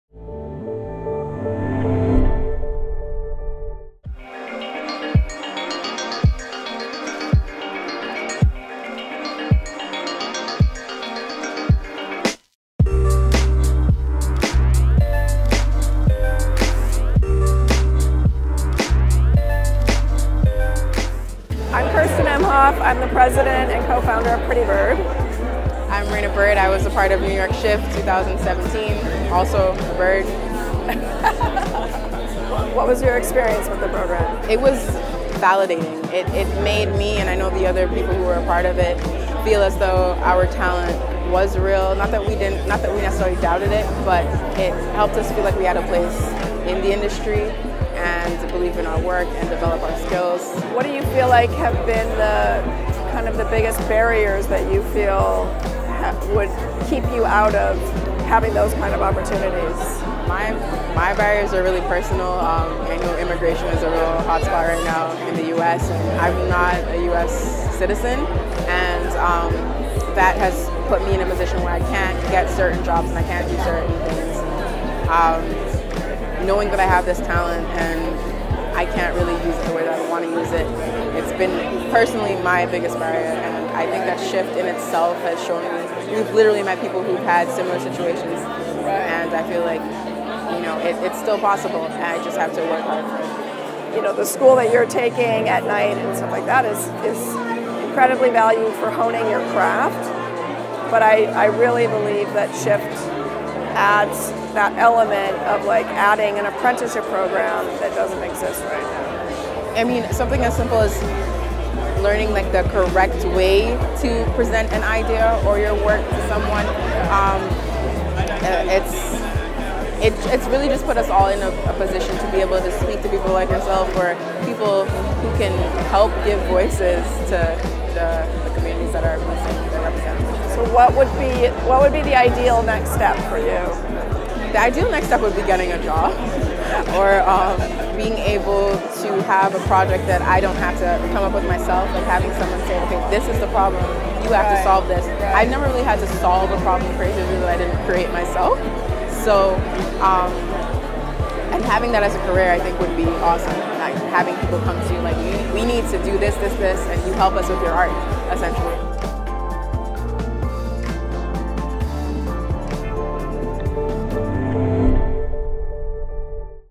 New Blood Shift New York 2017: Raena Bird in Conversation with Kerstin Emhoff
At the tail end of New Blood Shift New York we invited three Shifters to pair up with members of our newly appointed International Advisory Board to discuss creativity and the challenges of our industry. We were on hand to listen in as industry greats met our fresh crop of talent.

In this episode President & Co-Founder of Prettybird Kerstin Emhoff and Shifter Raena Bird talk about the importance of the Shift programme and creative validation. Most importantly, they touch upon the barriers to enter the creative industries as a young professional.

D&AD represents global creative, design and advertising communities and celebrates brilliance in commercial creativity.